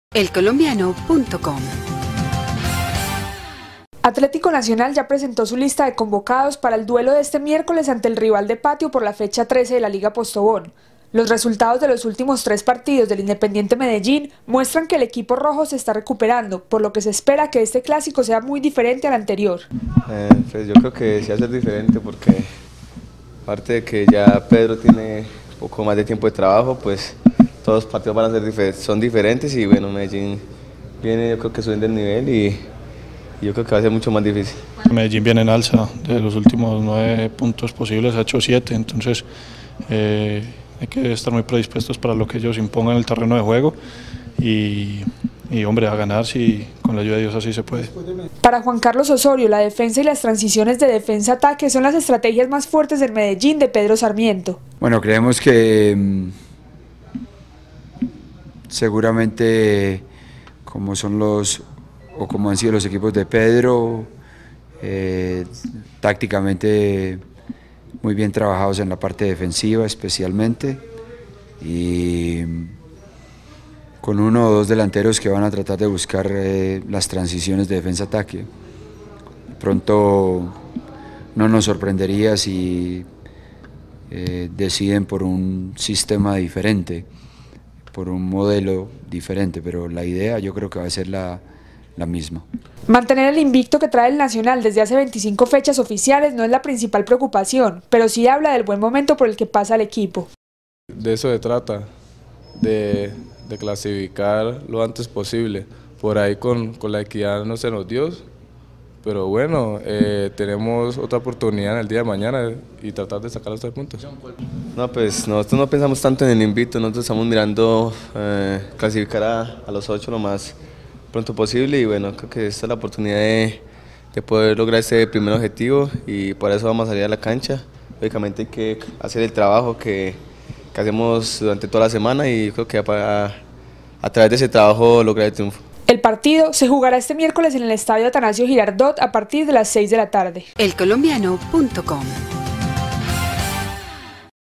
Nacional busca la clasificación en el Clásico para la vida
Una victoria en el clásico número 278 ante el Medellín le garantizaría al Atlético Nacional un cupo en los cuadrangulares semifinales. Por eso, el equipo dirigido por Juan Carlos Osorio espera conseguir los tres puntos y concentrarse en los próximos encuentros de Copa Postobón y Copa Sudamericana.